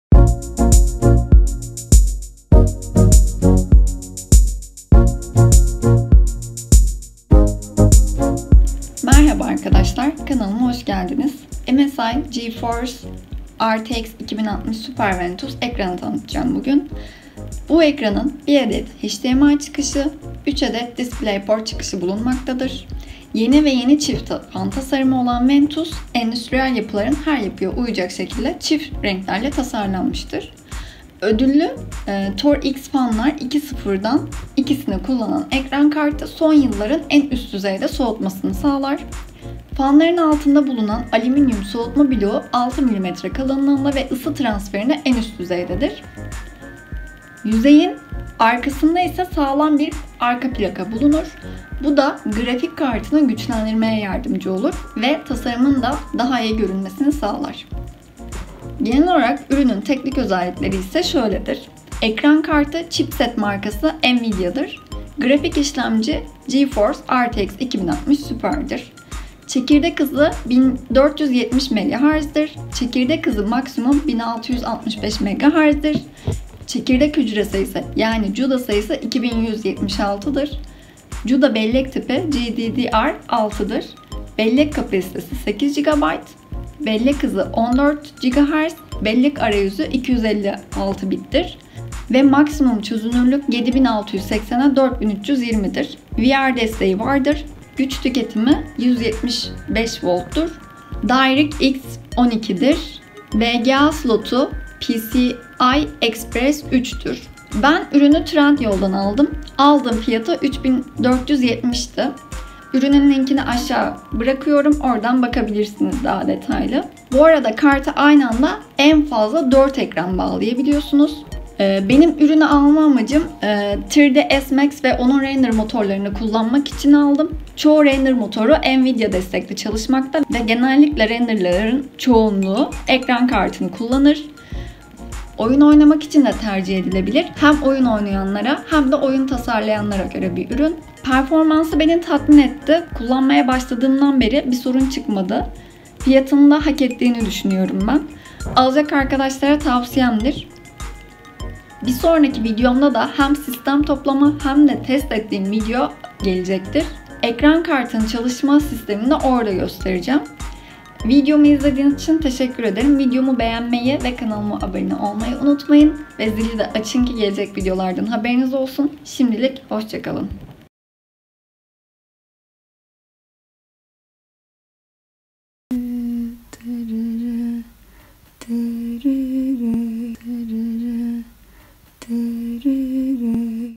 MSI GeForce RTX2060 SUPER VENTUS GP OC 8GB GDDR6 256Bit DX12 Nvidia Ekran Kartı / Süper Fiyat 👏 💪👍
MSI GeForce RTX 2060 SUPER VENTUS ekran kartını tanıtacağım.

1 adet HDMI  çıkışı 3 adet DP(displayport) çıkışı bulunmaktadır.

Yeni ve yeni çift fan tasarımı olan ventus, endüstriyel yapılarını her yapıya uyacak şekilde nötr renklerle tasarlanmış.

Ödüllü Torx fanlar 2.0 dan ikisini kullanan ekran kartı son yılların en üst düzeyde soğutmasını sağlar.

Fanların altında bulunan aliminyüm soğutma bloğu 6mm kalınlığında ve ısı transferinde en üst düzeydedir.

Bu yüzeyin arkasında ise sağlam bir arka plaka bulunur. Bu da grafik kartını güçlendirmeye yardımcı olur ve tasarımında daha iyi görünmesini sağlar.

Genel olarak ürünün teknik özellikleri şöyledir.
Ekran Kartı Chipset Marka: NVIDIA®
Grafik İşlemci: GeForce RTX 2060 SUPER
Çekirdek Hızı: 1470 MHz
Çekirdek Hızı Maks.:1665 Mhz
Çekirdek Hücre Sayısı (Cuda sayısı):2176
Cuda Bellek Tipi: GDDR6
Bellek Kapasitesi: 8GB
Bellek Hızı: 14.0 GHZ
Bellek Arayüzü:256 Bit
1 adet HDMI ve 3 adet DisplayPort çıkışı vardır
Maks. Çözünürlük 7680 x 4320
Soğutma:Aktif
Soğutma Fan Sayısı 2
Vr Desteği:Var
Güç Tüketimi: 175 W
Direct X12
VGA Slot: PCI Express 3.0
OpenGL 4.5